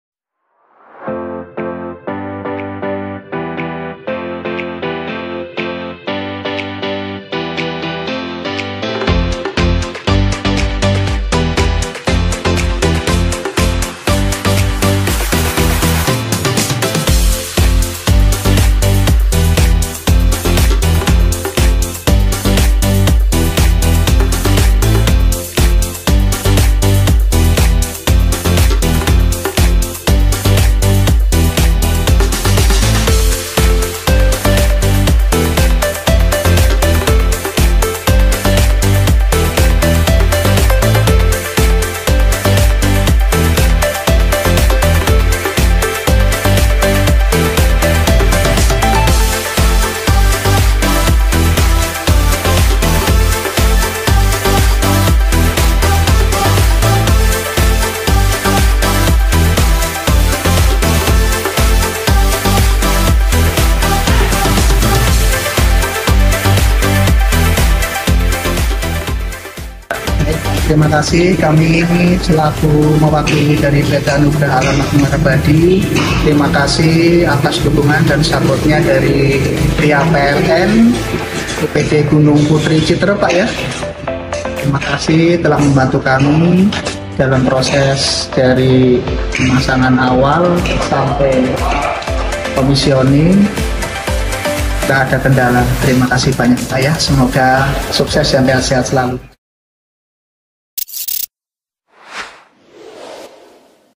PLN ULP Citeureup 5 Tahun terakhir Kepemimpipinan Erick Thohir berhasil Realisasikan Sambung Baru
PLN UP3 Gn. Putri dalam 5 Tahun terakhir Kepemimpipjnan Erick Thohir berhasil Realisasikan Penyambungan Baru PT Anugrah Alam Makmur Abadi

Electrizen,,

Pada hari selasa, tanggal 25 Juni 2024 PLN ULP Citeureup telah berhasil melakukan pemasangan tambah daya PT Anugrah Alam Makmur Abadi dari daya sebelumnya 11.000 VA menjadi 82.500 VA.

PT Anugrah Alam Makmur Abadi merupakan sebuah perusahaan yang bergerak di Industri pembuatan Buah dan Sayuran Pure ataupun Konsentrat yg menggunakan bahan-bahan pertanian lokal. Mereka mengolah bahan-bahan pertanian lokal dengan kualitas terbaik untuk dapat dipasarkan kepada calon-calon konsumen. Seiring dengan meningkatnya permintaan pasar terhadap hasil olahan PT Anugrah Alam Makmur Abadi ini, membuat mereka membangun plant baru di wilayah Citeureup.

PT Anugrah Alam Makmur Abadi menggandeng PT PLN (Persero) untuk mendukung kebutuhan penambahan daya dari plant baru mereka di wilayah Karanggan Muda, Citeureup. Permohonan Penambahan Daya ini tentunya akan meningkatkan produksi mereka.

Semoga dengan dilaksanakannya permohonan tambah daya tersebut dapat memberikan produksi yang maksimal kepada pelanggan dan juga PLN ULP Citeureup terus berkomitmen memberikan yang terbaik kepada seluruh pelanggan.